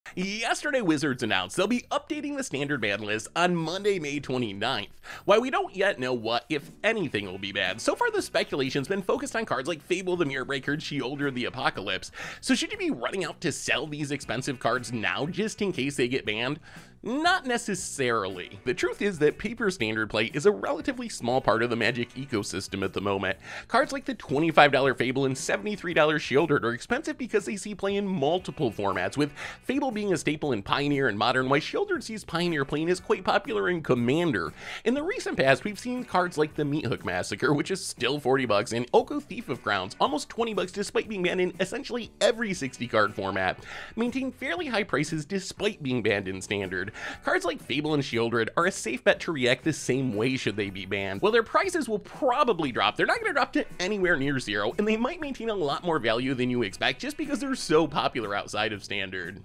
Should You Sell Fable or Sheoldred in Case they Get Banned on May 29th? | MTG
Wizards announced yesterday that they'll be updating the Standard ban list on May 29. While we don't yet know what, if anything, will be banned, speculation has focused on Fable of the Mirror-Breaker and Sheoldred, the Apoclypse. Should you sell these cards now in case they get banned in Standard? Let's discuss!